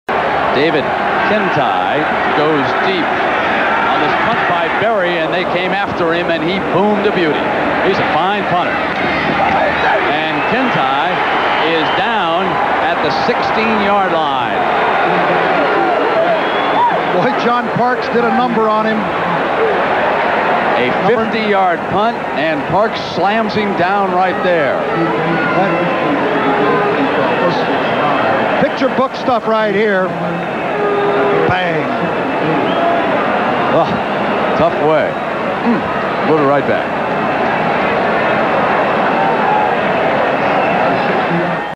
Louis Berry's 50 Yard Punt Miami versus FSU 1986
Punters are taught to use the sideline as an additional defender. This 50 yard punt during the Miami versus Florida State game in 1986 exemplifies this fundamental football lesson.